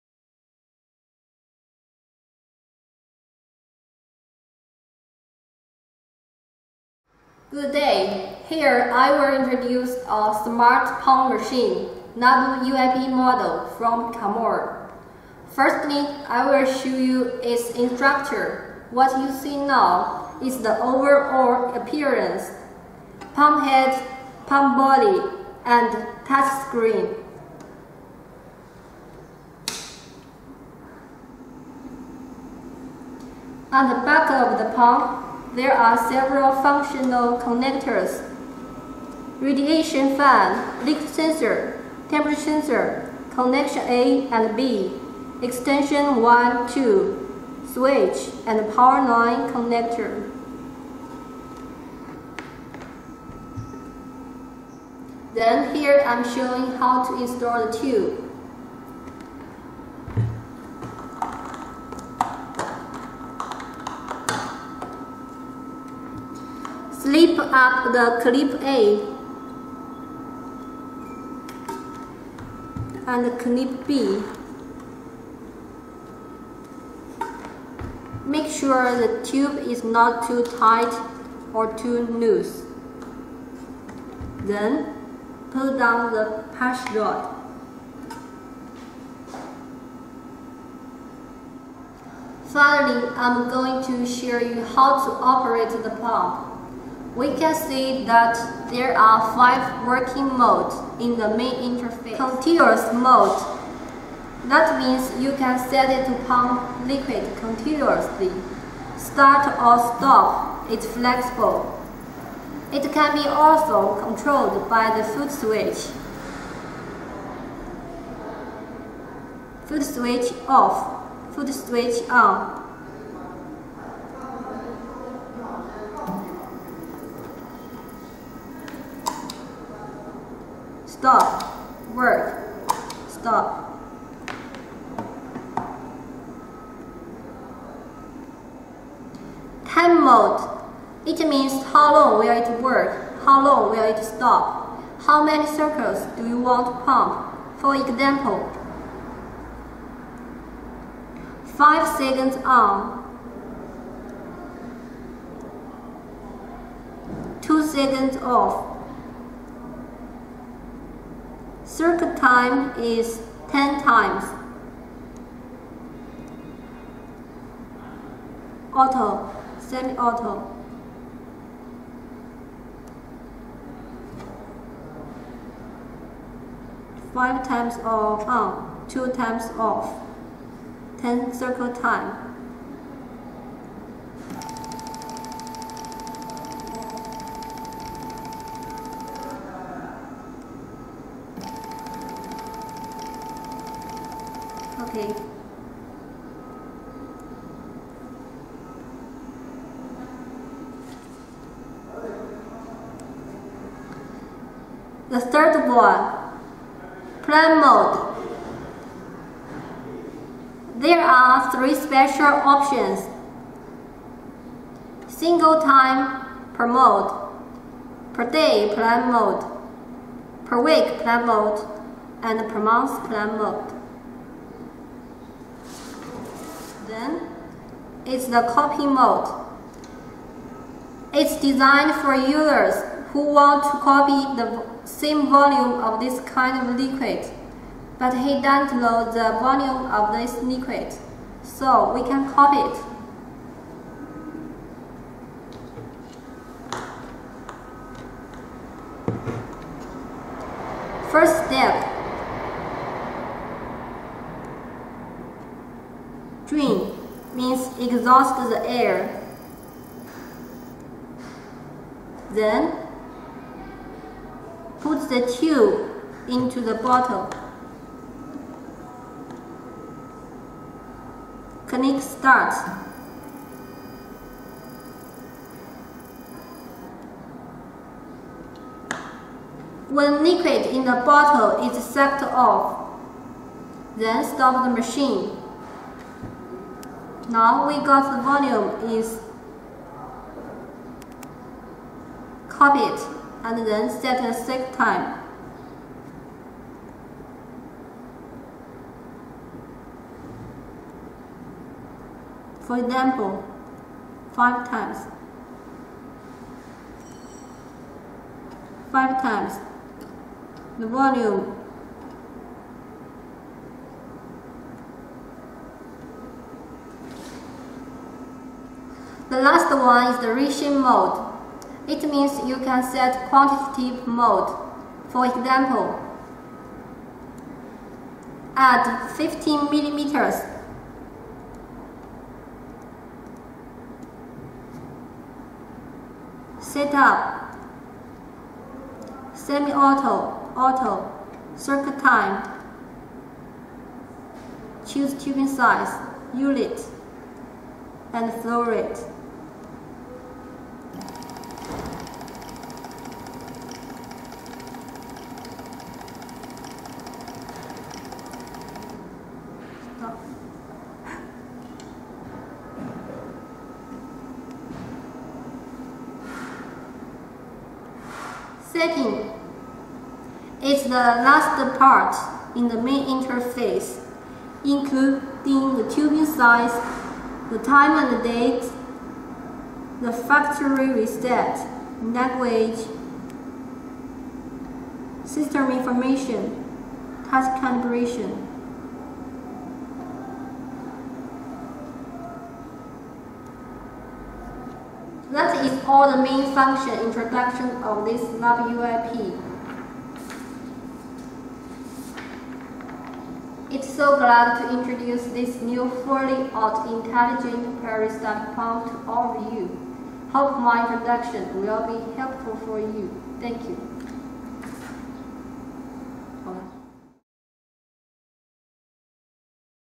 Kamoer UIP Laboratory Peristaltic Pump (English version)
UIP from Kamoer fluid tech(Shanghai)Co.,Ltd makes dispensing easy. With different applications and different tubing, UIP can dispense into any lab ware.